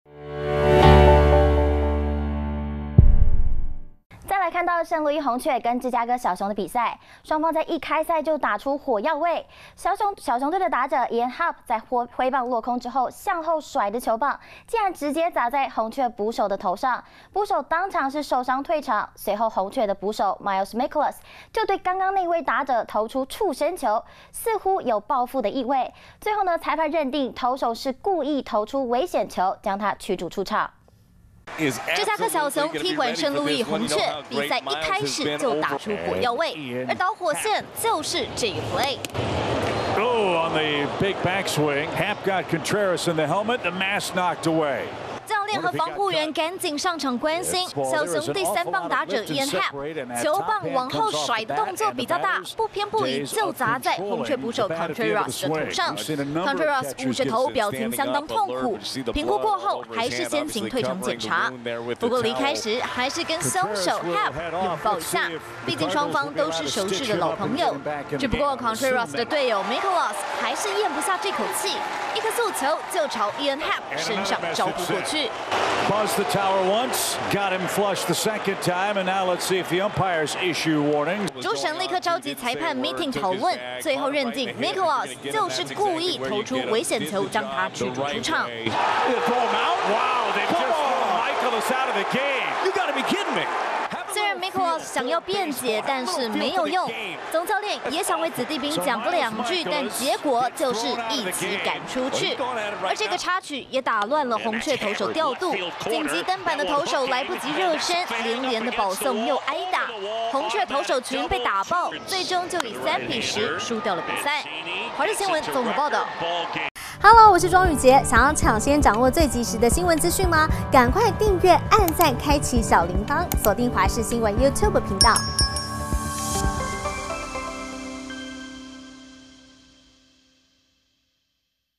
隊友遭球棒砸破頭 紅雀投手報復觸身球遭驅逐｜華視新聞 20230728
來看到聖路易紅雀和芝加哥小熊之戰，雙方在一開賽就打出火藥味！小熊隊打者哈普(Ian Happ)在揮棒落空後，向後甩的球棒，竟然直接砸在紅雀捕手的頭上，捕手當場受傷退場；隨後紅雀投手麥可拉斯(Miles Mikolas)立刻投出觸身球，似乎有著報復意味。最後裁判認定投手是故意丟出危險球，將他驅逐出場。